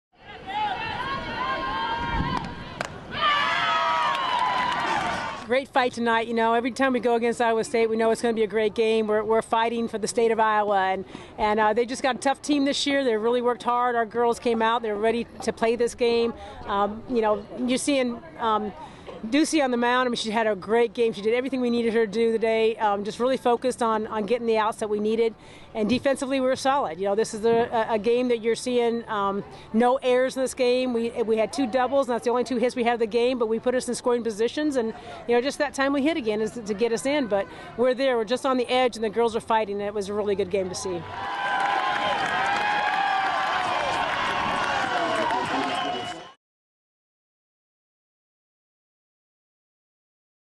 Hawkeyes Battle in Iowa Corn Cy-Hawk Series
The Iowa Hawkeyes faced cross state rival Iowa State on the diamond in the Iowa Corn Cy-Hawk Series.